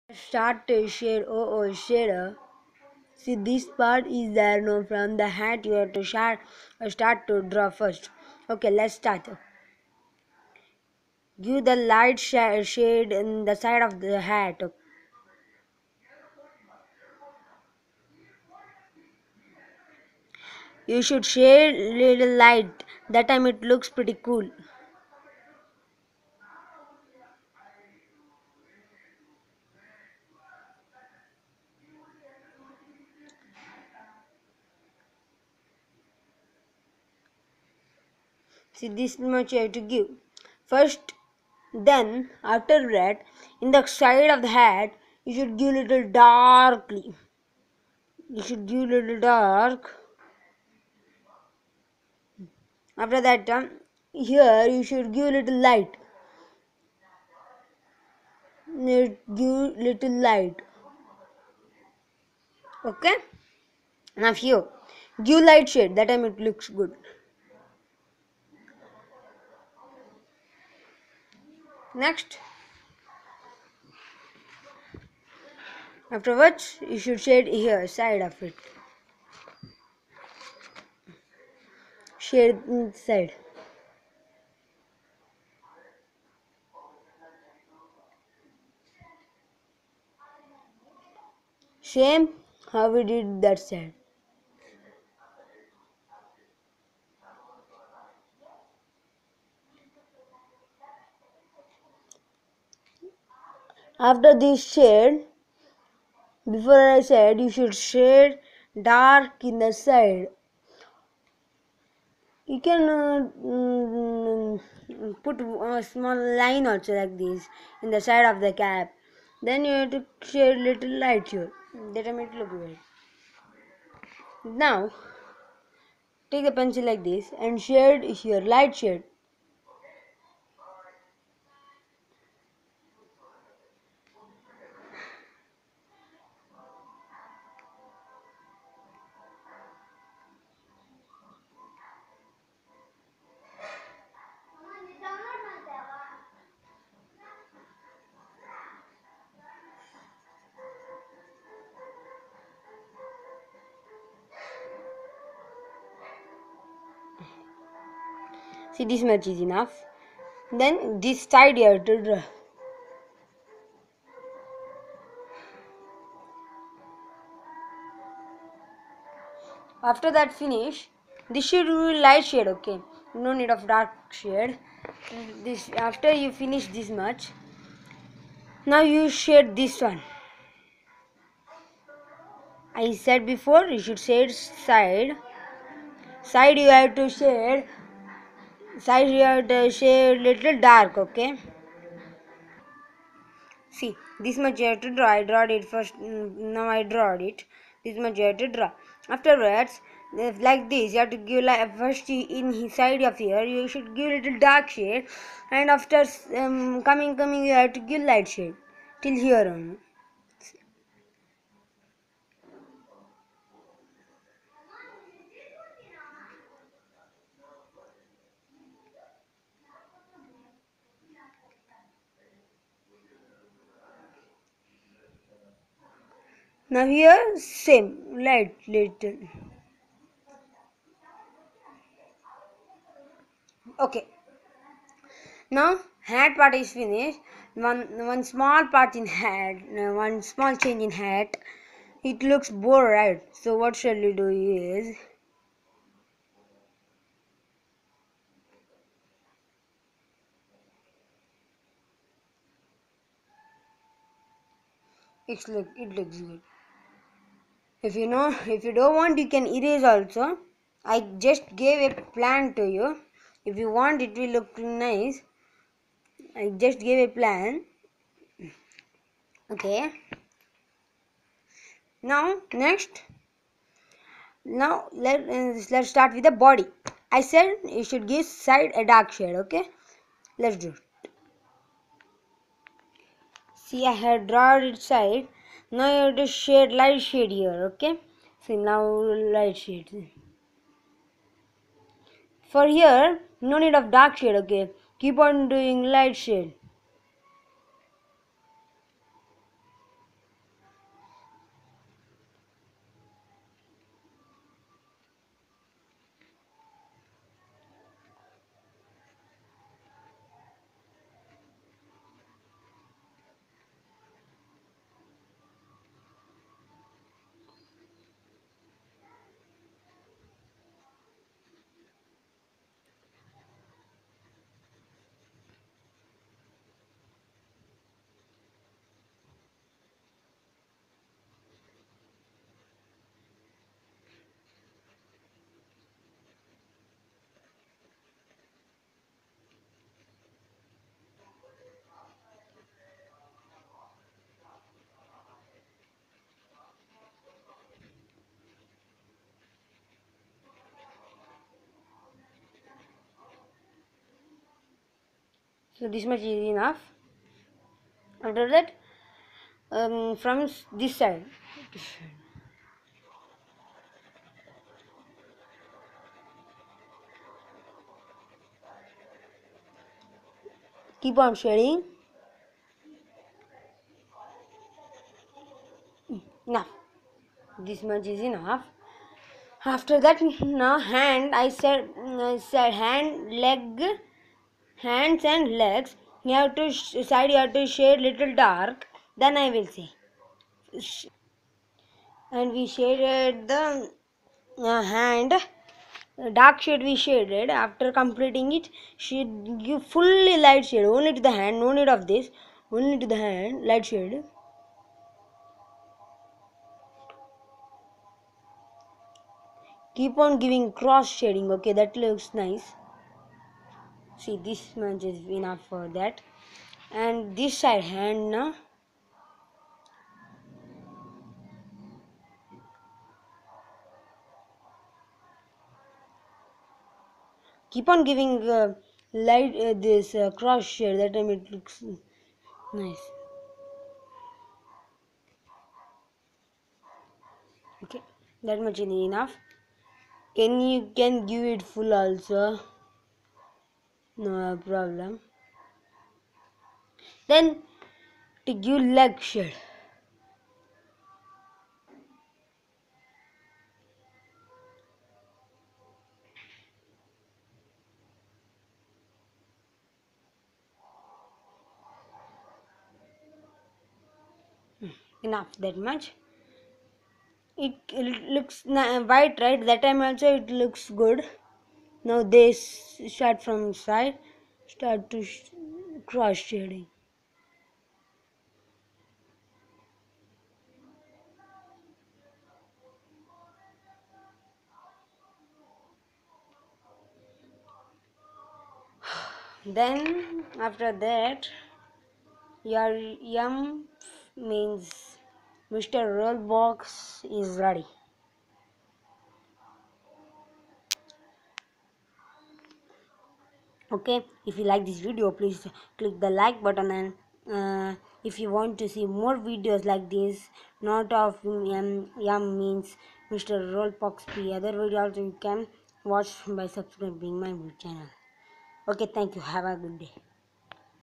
How to shade Mr.p from Rolbox ooooooooooo shader
You have to start from head and end by hands and legs